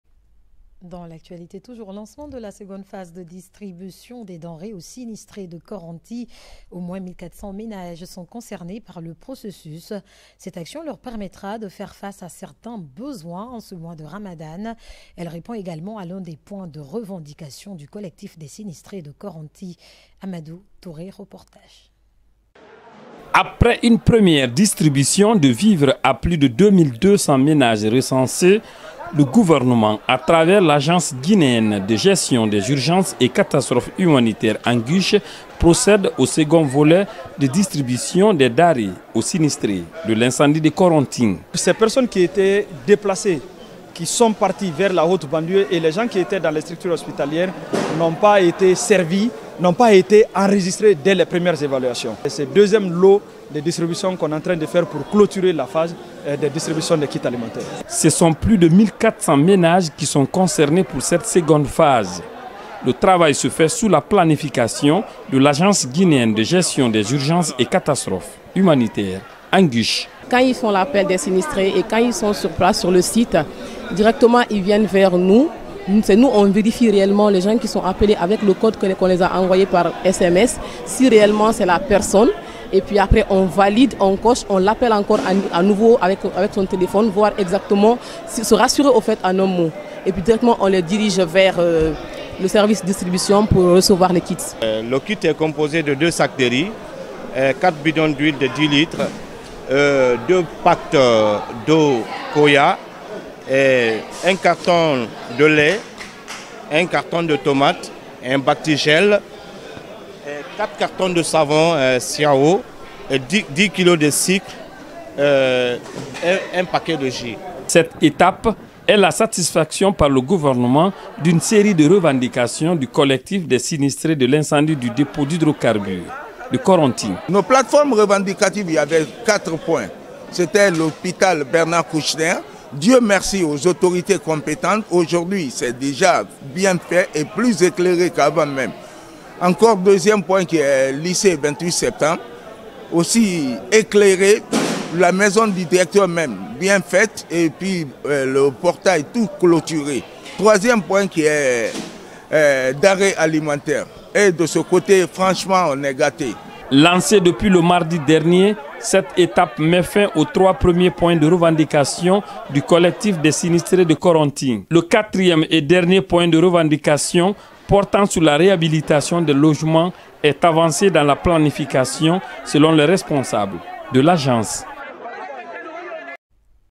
HUMANITAIRE / ANGUCH Des vivres pour les sinistrés de Kaloum. A suivre sur la RTG GUINEE 224
"VOUS SERVIR EST NOTRE MISSION"